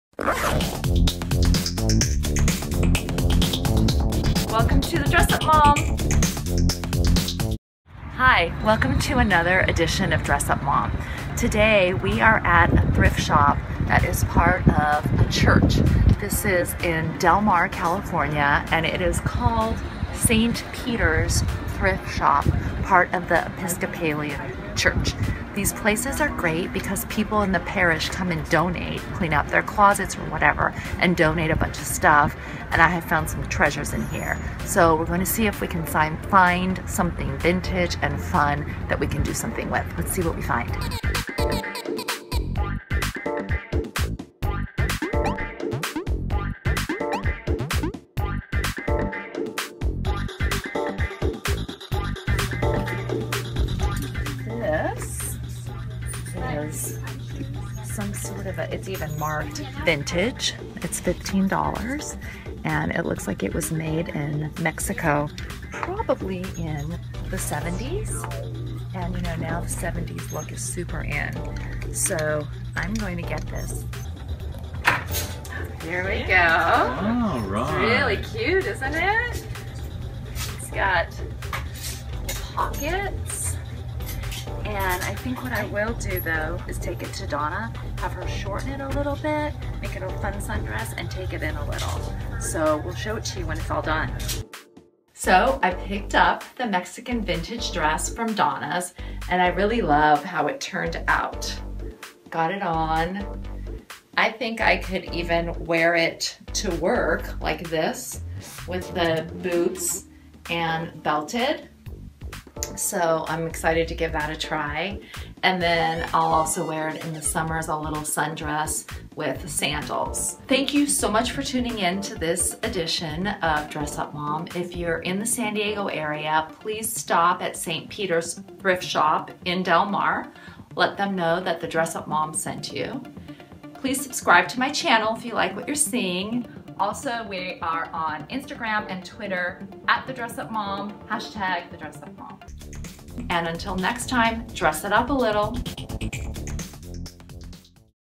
How to Update a Vintage Mexican Dress ~ The Dress Up Mom Shops at St. Peter's Thrift Shop
I'm shopping at St. Peter's Thrift Shop in Del Mar, California. The store supports the ministry of St. Peter's Episcopal Church, and congregation members donate clothes and home goods to the shop. I've found some treasures here in the past, and today is no different. I bought a vintage Mexican dress from the 1970s for $15! It is so colorful and fun. I do want the length to be shorter, and it needs to be taken in a bit. A quick trip to Donna's Tailor Shop gives it a more modern look. Now I can definitely wear this dress to work with a belt and boots. I know I'll also wear it a ton this summer as a sundress with sandals. If you are in the area, please visit St. Peter's Thrift Shop and tell them The Dress Up Mom sent you. You're bound to find something fabulous.

I really appreciate it! Until next time, have fun & dress it up a little. ~ Rajka Hayden, The Dress Up Mom

St. Peter's Thrift Shop - Del Mar, CA

Donna's Tailor Shop in Encinitas, CA
I trust Donna to fit & reconstruct any garment.

~ Social Media ~